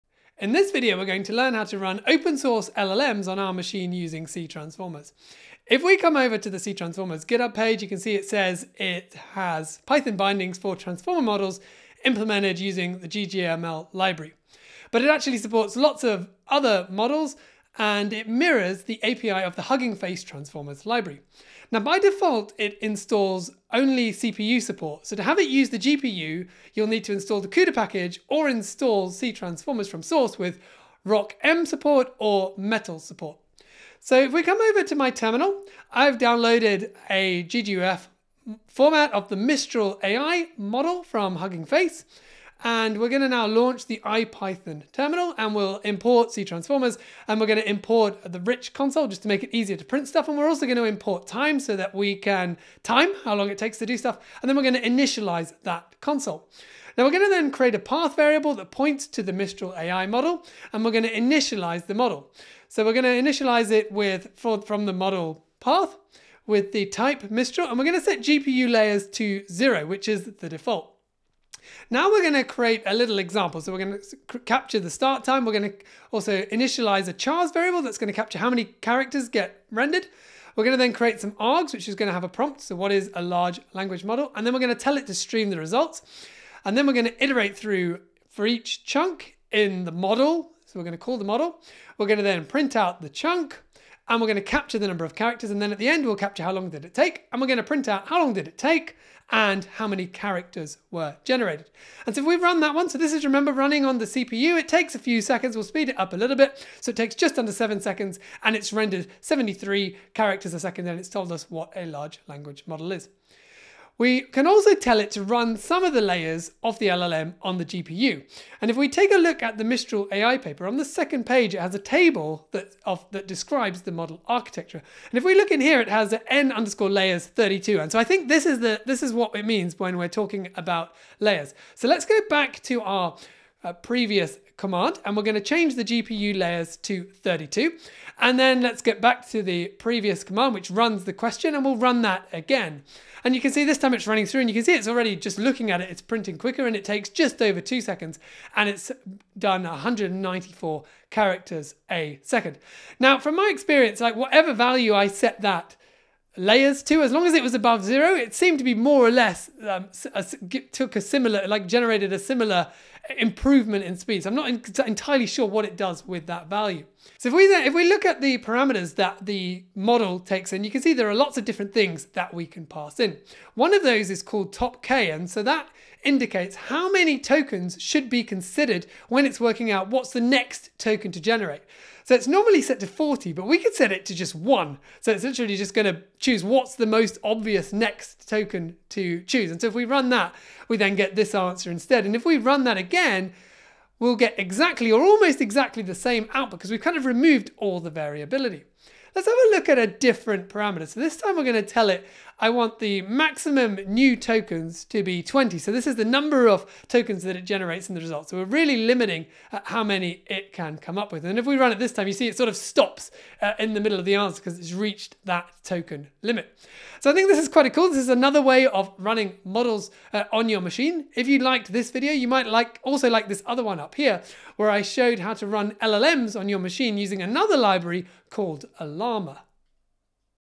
LLMs on your own machine using CTransformers
In this video, we'll learn how to run open-source Large Language Models (LLMs) on our machines using C Transformers. We learn how to run the Mistral AI GGUF model with both CPU and GPU support and have a look at how parameters vary the output generated.